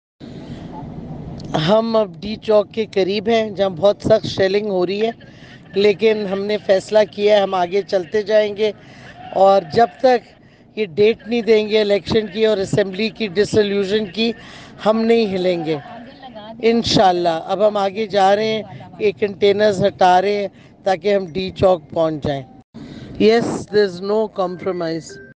Exclusive: Shireen Mazari's Strong Message On Reaching D-Chowk
PTI leader, Shireen Mazari spoke exclusively to ASM Today from Imran Khan's container just before reaching D-Chowk.